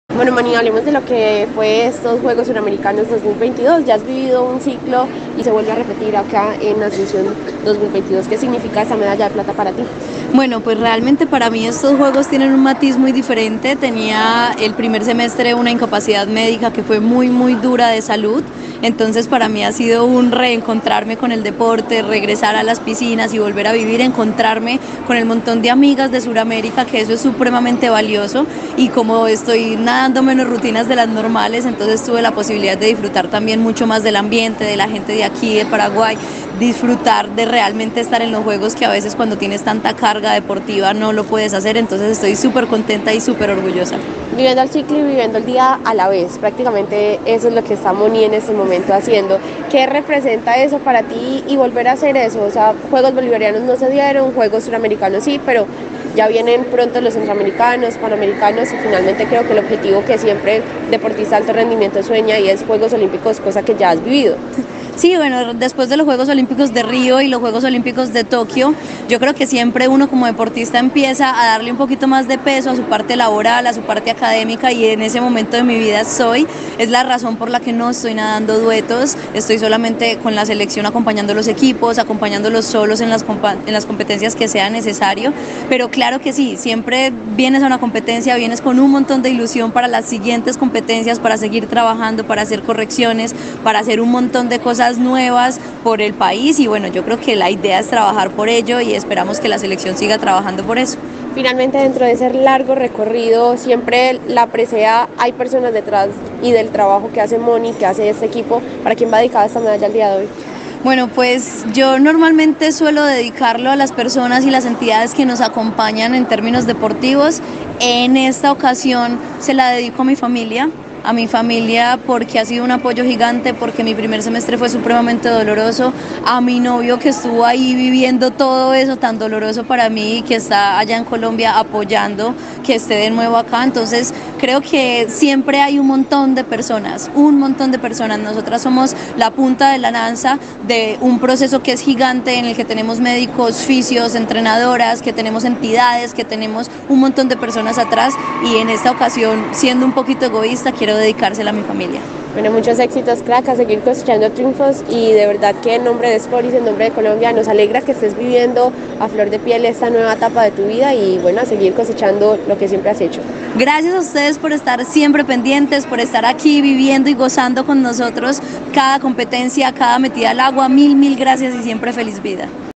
Mónica Arango subcampeona 🥈🇨🇴 por equipos natación artística | Juegos Suramericanos Asunción 2022
Los Juegos Suramericanos, es el evento deportivo multidisciplinario más importante de Suramérica y se realiza cada 4 años , 10 países de América del Sur y 5 países de Centroamérica y el Caribe.